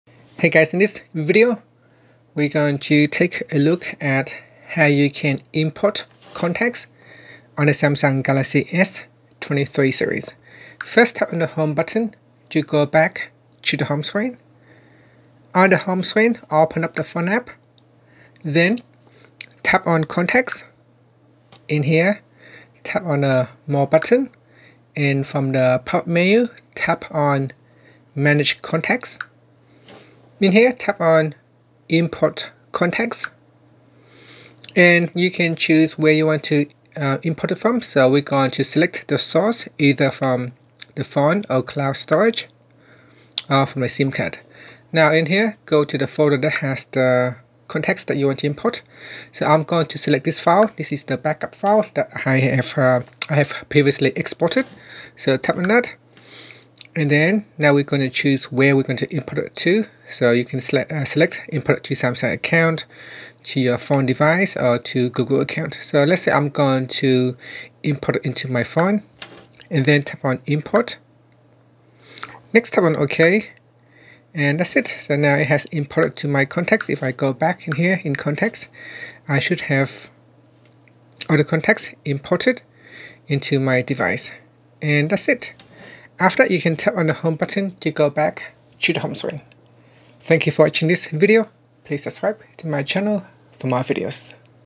Galaxy S23's: How to Import Contacts
Learn how you can import Contacts on the Samsung Galaxy S23/S23+/Ultra.

You can import contacts to your Galaxy S21 using several methods:

Google Account: If you have your contacts stored in your Google Account, you can easily import them to your Galaxy S23 by signing in to your Google Account on your phone. This will automatically sync your Google Contacts to your phone.

SIM card: If you have contacts saved on your SIM card, you can import them to your Galaxy S21 by going to Contacts - Settings - Import/Export - Import from SIM card. Your phone will automatically detect the contacts saved on your SIM card and allow you to select which ones you want to import.

vCard file: If you have a vCard file (.vcf) containing your contacts, you can import them to your Galaxy S21 by going to Contacts - Settings - Import/Export - Import from storage. You will need to navigate to the location where the vCard file is stored and select it.

Other accounts: If you have contacts saved in other accounts, such as Outlook or Facebook, you can import them by signing in to those accounts on your Galaxy S21 and syncing your contacts.

Overall, importing contacts to your Galaxy S23 is a simple and straightforward process. Depending on where your contacts are stored, you can choose the method that works best for you and easily transfer your contacts to your new phone.